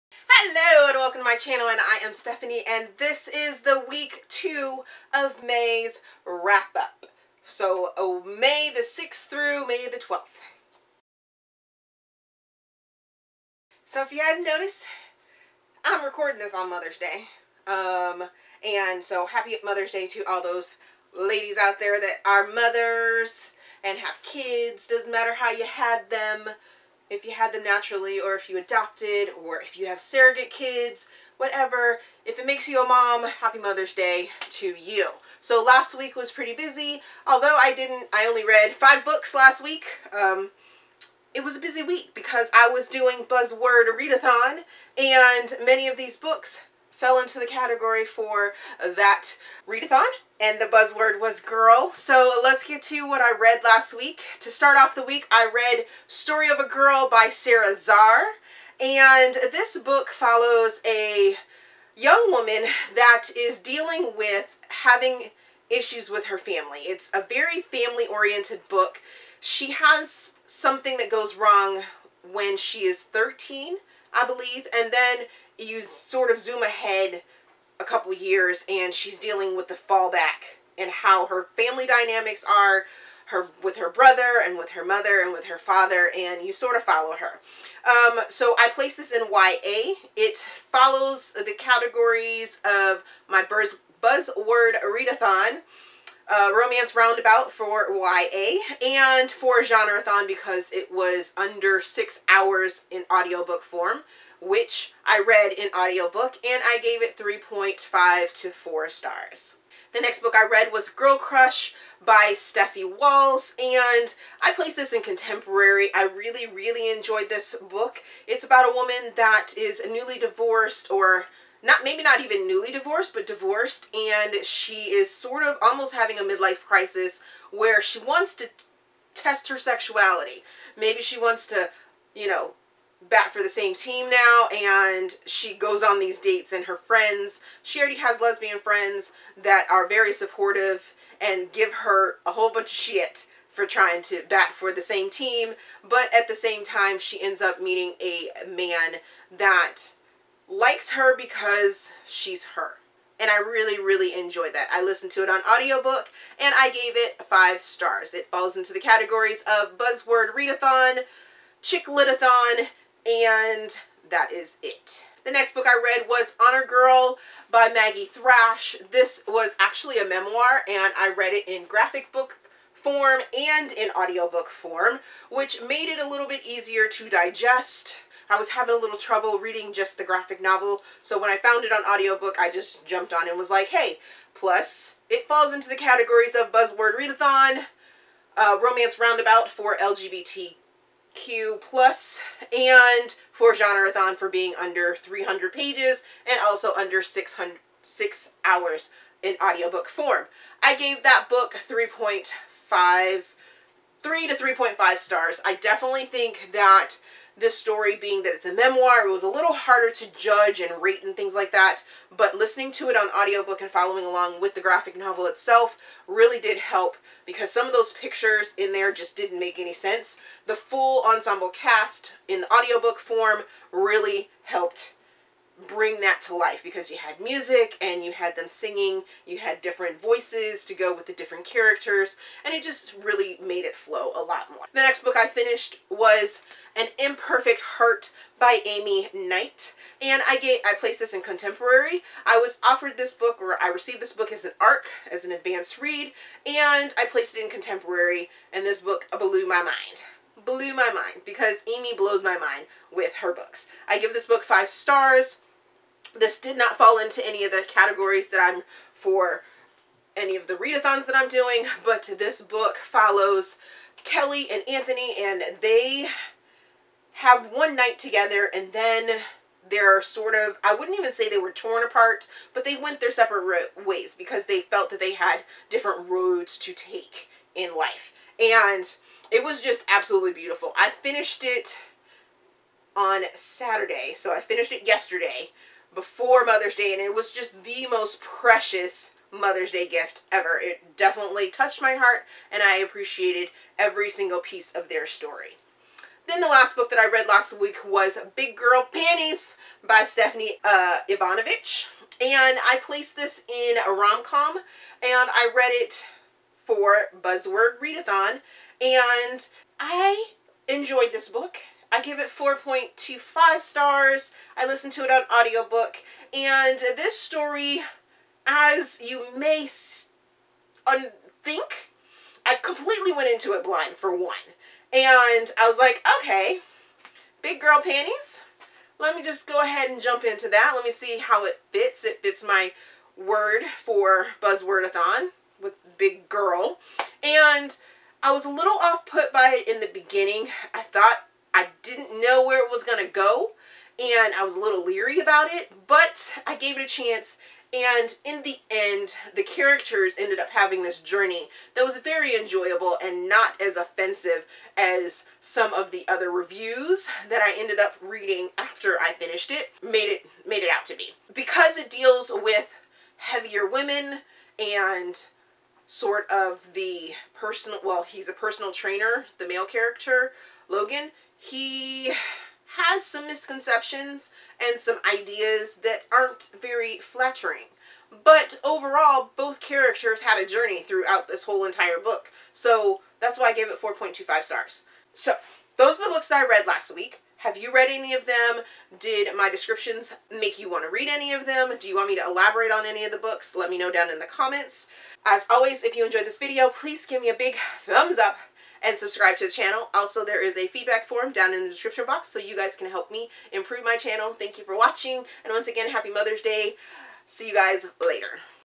May 6-12 Wrap Up | buzzwordreadathon Wrap Up
HAPPY MOTHER’S DAY!! Here are the books that I read in the second week of May.
Want to see a specific review…OPEN ME

Books mentioned:
Story of a Girl by Sara Zarr 00:55
Girl Crush by Stephie Walls 1:50
Honor Girl by Maggie Thrash 2:47
An Imperfect Heart by Amie Knight 3:56
Big Girl Panties by Stephanie Evanovich 4:59